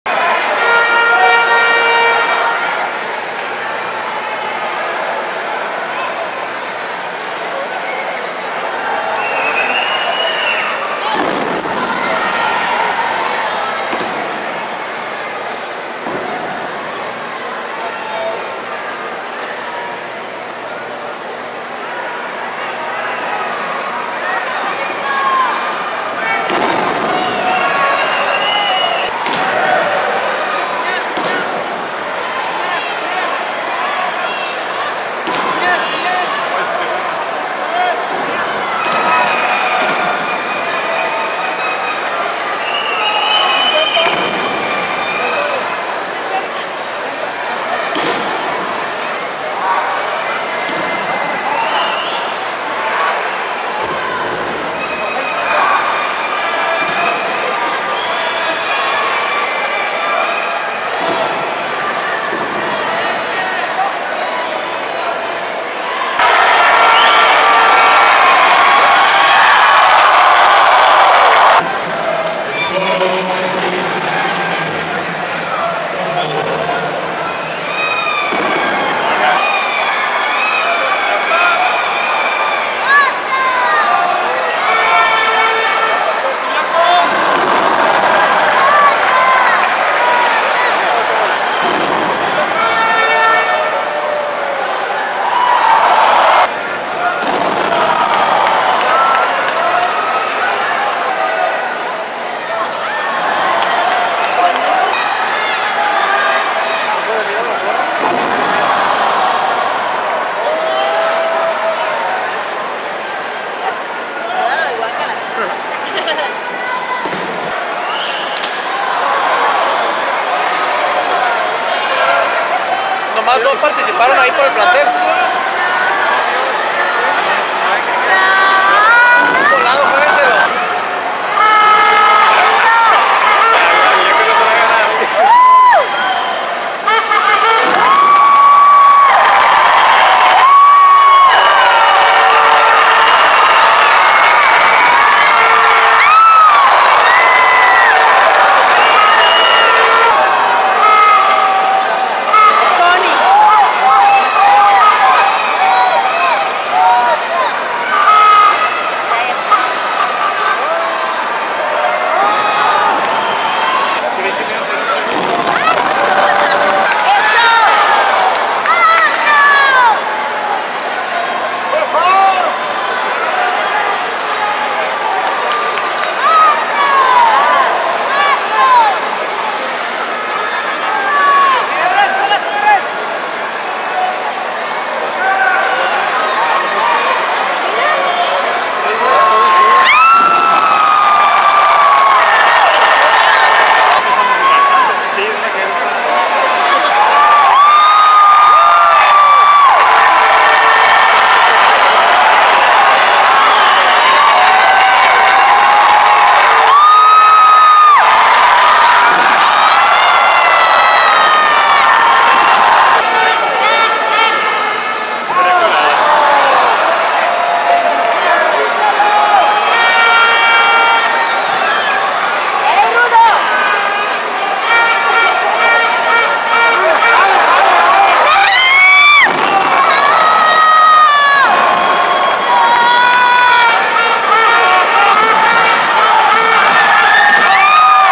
Histeria vs Enigma [15 jul 16] Auditorio de Tijuana
Un mano a mano por el campeonato ALLM en la noche del 15 de julio de 2016 en el auditorio de Tijuana por parte de Promociones el Cholo de Tijuana.